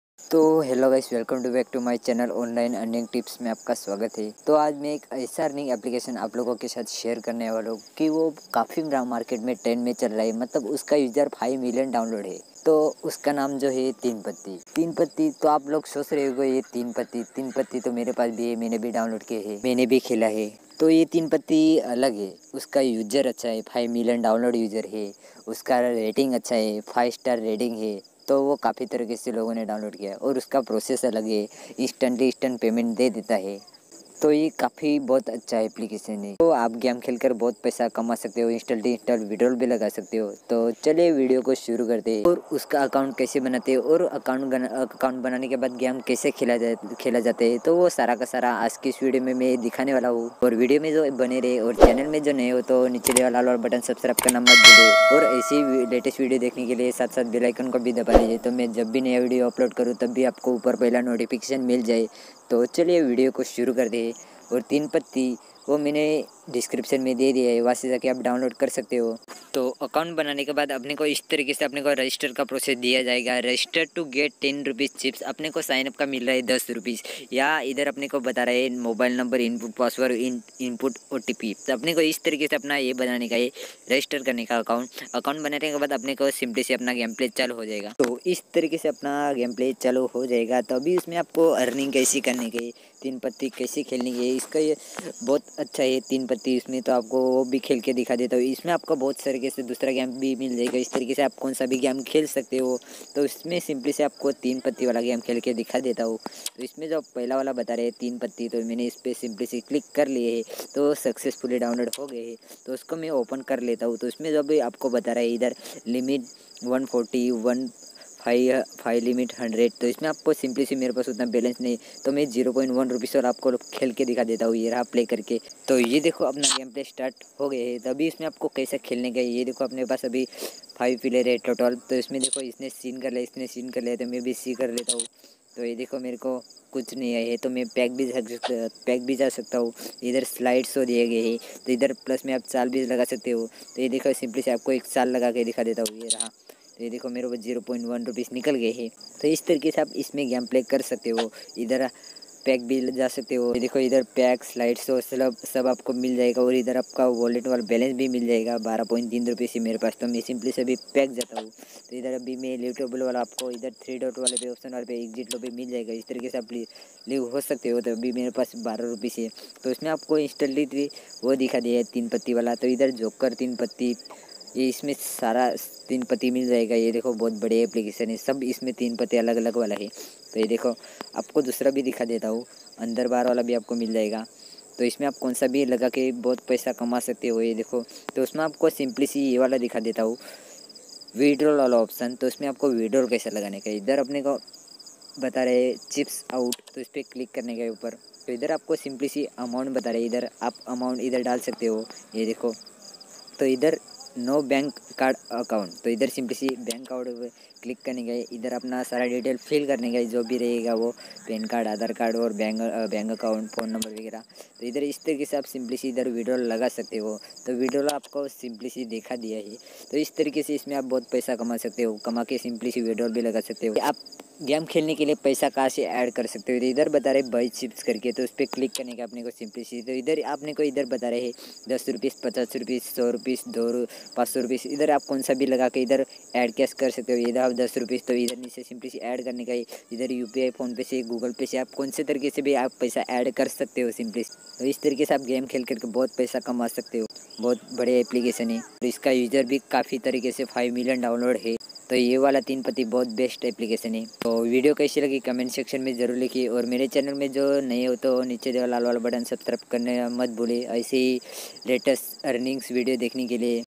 teen patti real cash game | teen patti win kaise khele | teen patti win withdraw | teen patti to
Hello Guys
                  आज की इस व्हिडिओ मे लेकरं आया हू एक ऐशी Teen Patti App के बारे मे Best App 5 Milions User Dawonload तो Teen Patti कैसे खेले और पैसे कैसे कमाये

🔵और मे ऐशी ही व्हिडीओ लेके आता हू EARNINGS के बारे मे 🔵